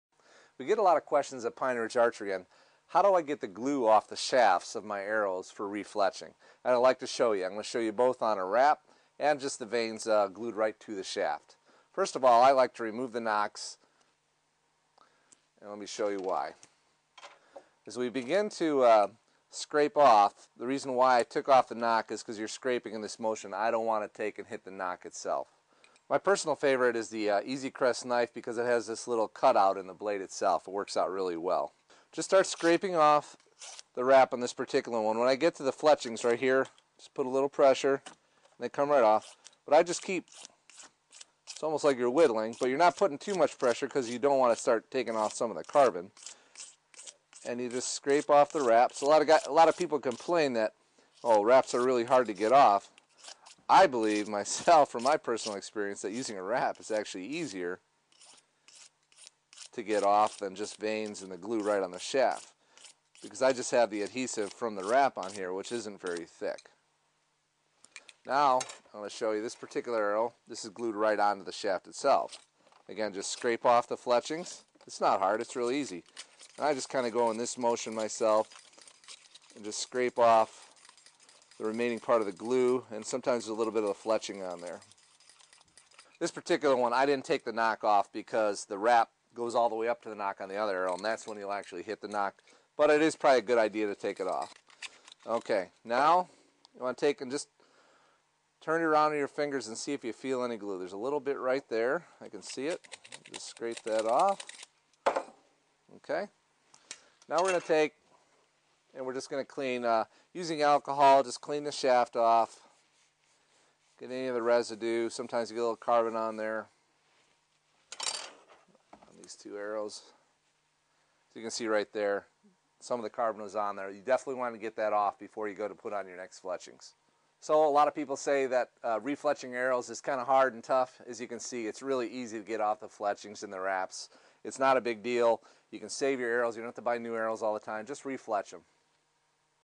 ReFletch 2009
Re-Fletching arrows is easy.  Jim Broberg shows you how easy it is!